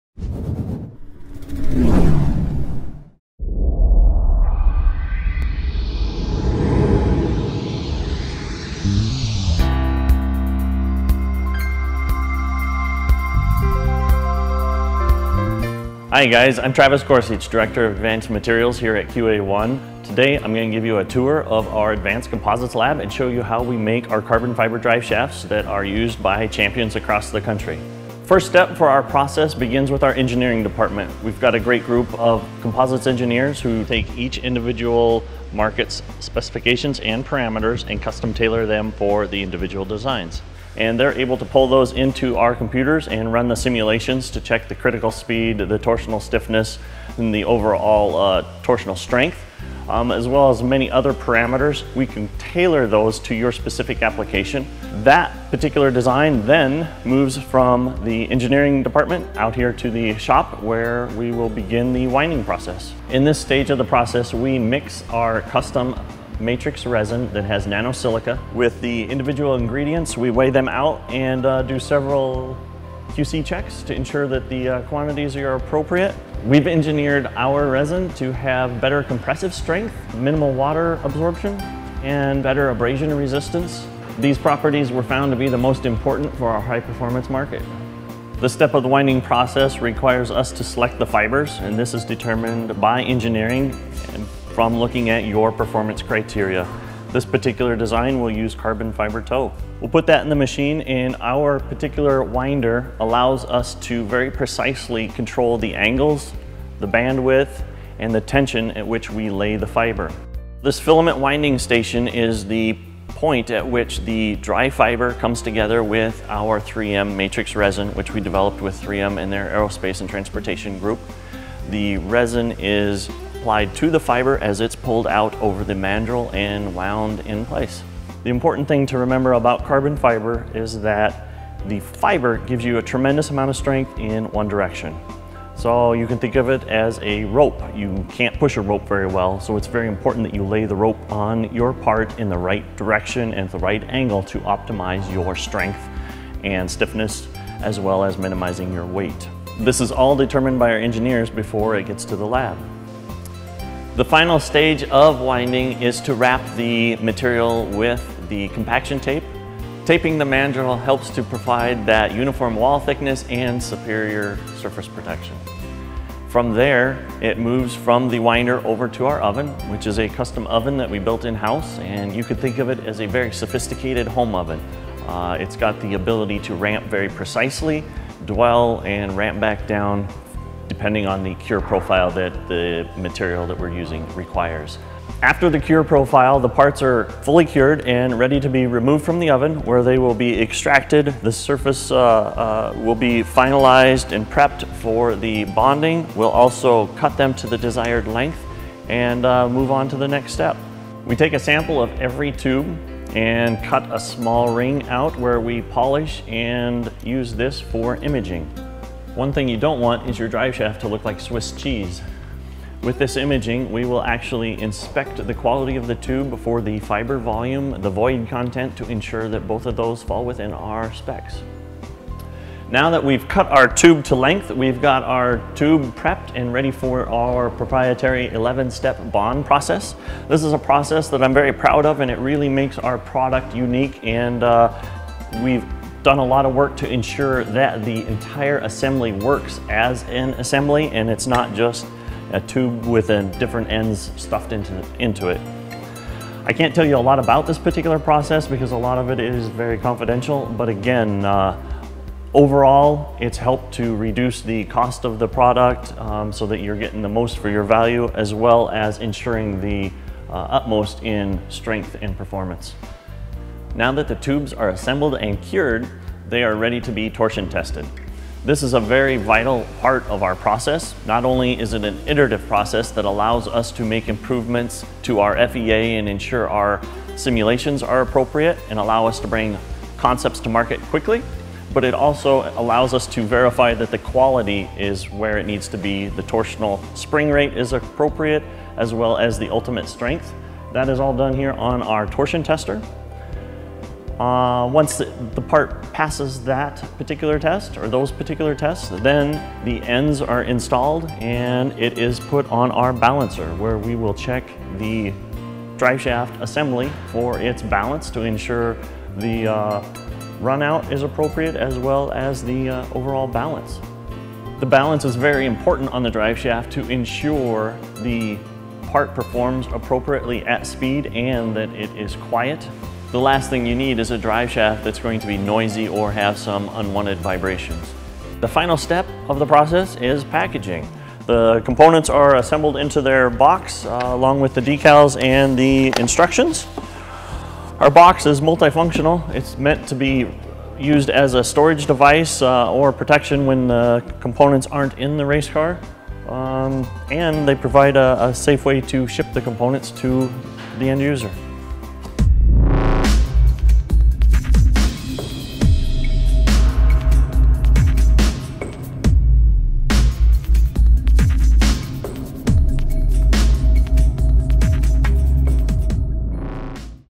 The Construction of QA1 Carbon Fiber Driveshafts
QA1 is driven to pursue its goal of making the finest performance suspension and driveshaft components constantly raising the bar on quality.
Watch this video to learn more about QA1 Carbon Fiber Driveshafts!